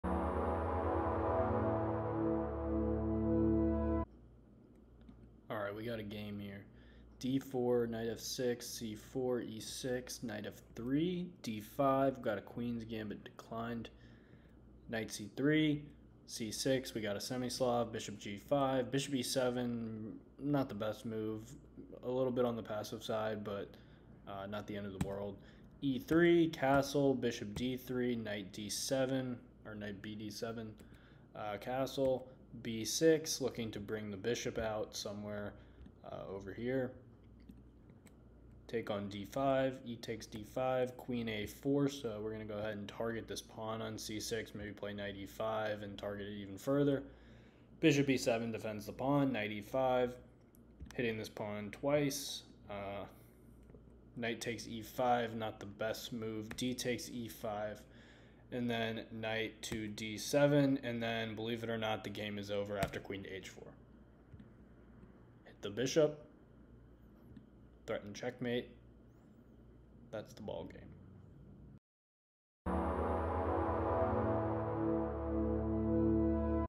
Jan Timman's Sneaky Opening Trap (Semi-Slav Defense)
I came across this game, and it really is a beautiful trap as it looks rather harmless for black. However, the game is over instantly by the beautiful Qh4!! 1-0